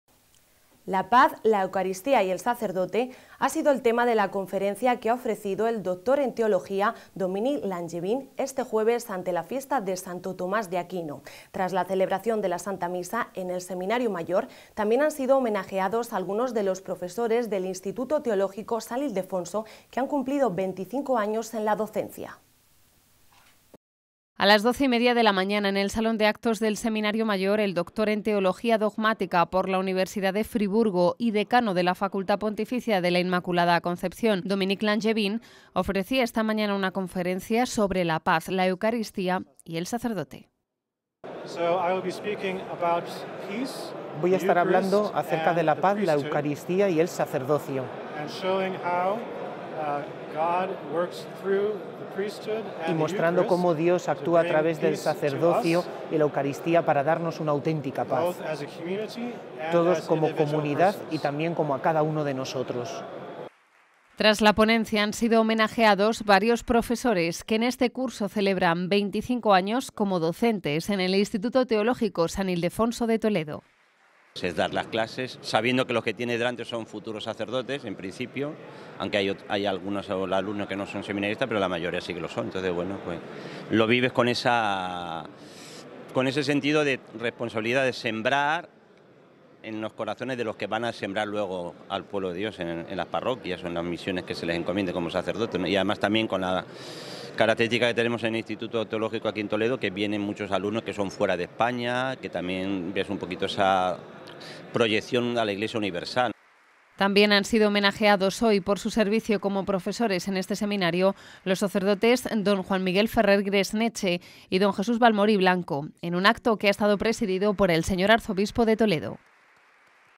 25 años de docencia
“La Paz, la Eucaristía y el sacerdote” ha sido el tema de la conferencia que ha ofrecido el doctor en Teología Dominique Lanllevin este jueves ante la fiesta de Santo Tomás de Aquino. Tras la celebración de la Santa Misa, en el Seminario Mayor, también han sido homenajeados algunos de los profesores del Instituto Teológico San Ildefonso que han cumplido 25 años en la docencia.